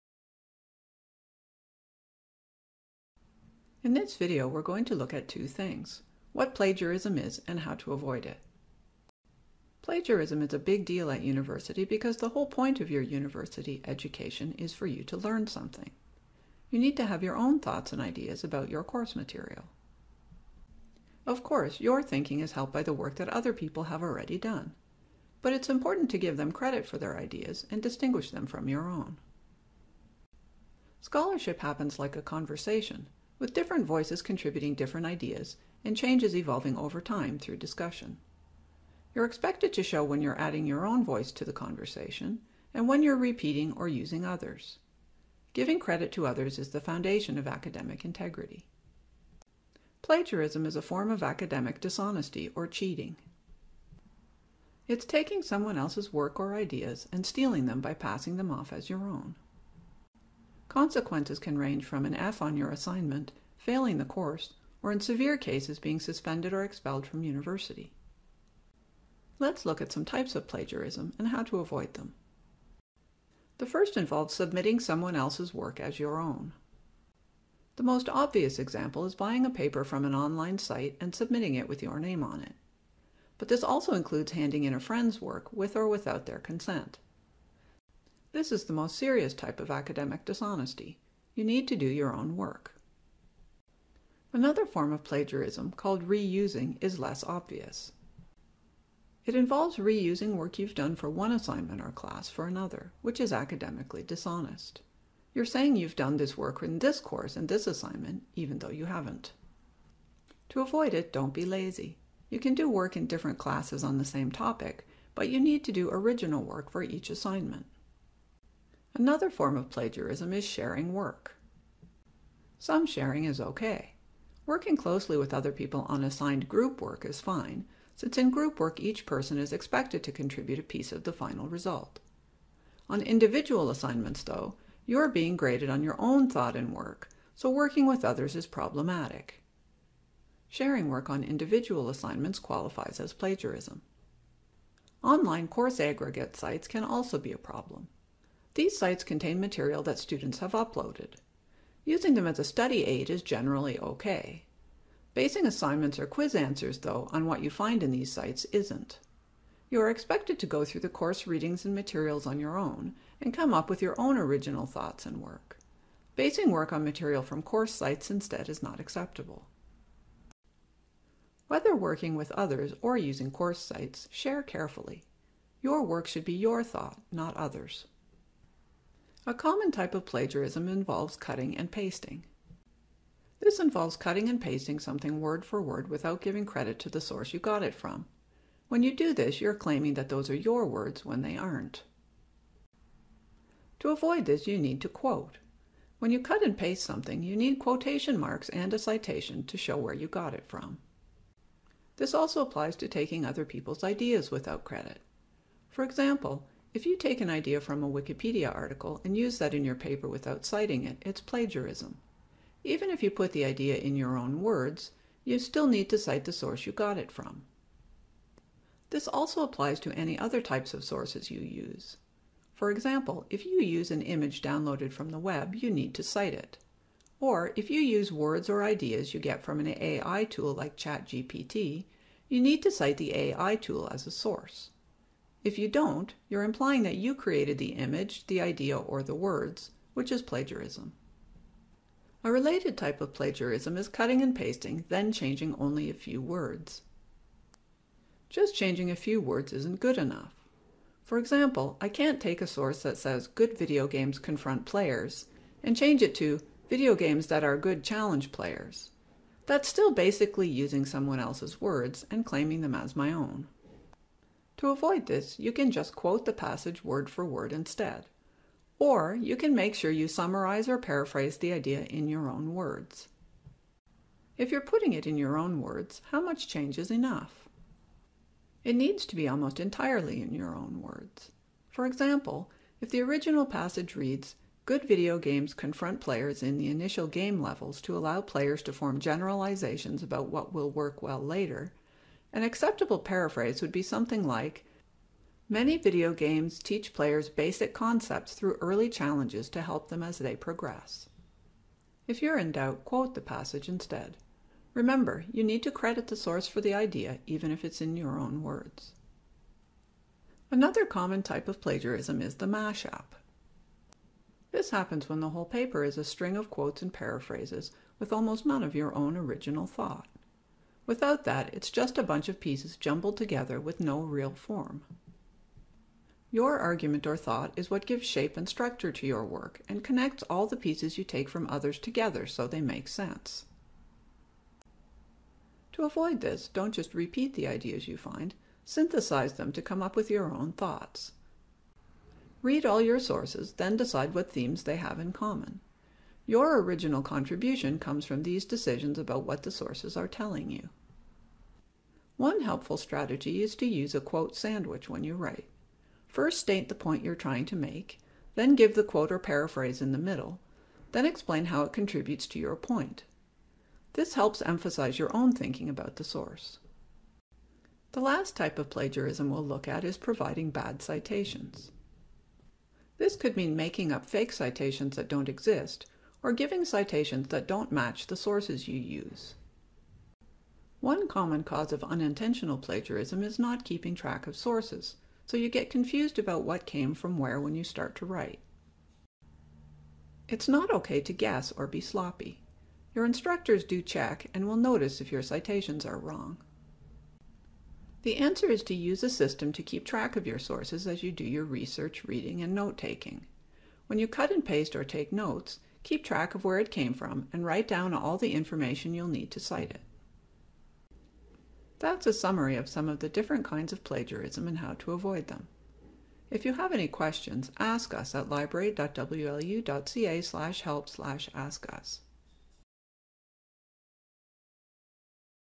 Understanding plagiarism
Types of plagiarism and how to avoid them.